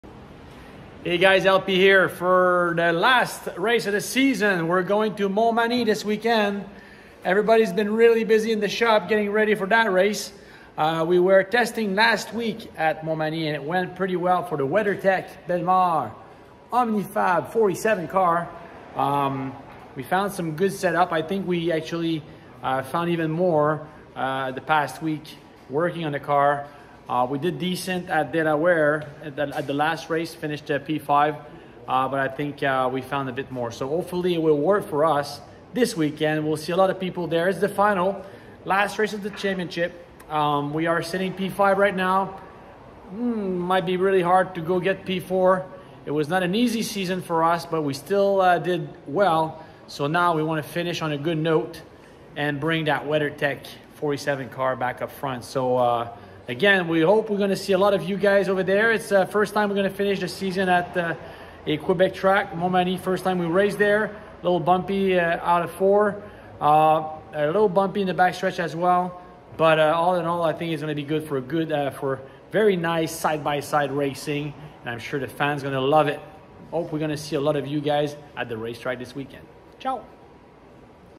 Quebec will host the NASCAR Canada Series Finale
For the first time in history, the NASCAR Canada Series will hold the season finale in the province of Quebec! L.P. will look for a strong performance in his home province.